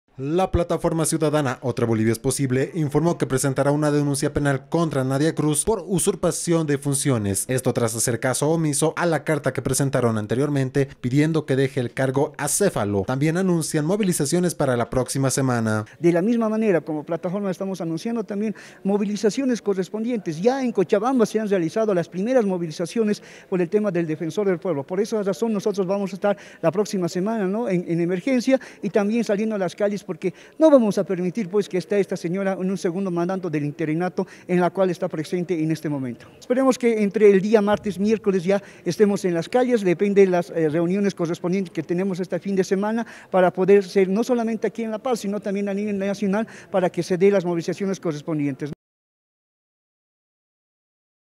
PLATAFORMA CIUDADANA PRESENTA DEMANDA PENAL CONTRA NADIA CRUZ
Plataforma Ciudadana presenta demanda penal contra Nadia_Cruz y anuncia movilizaciones.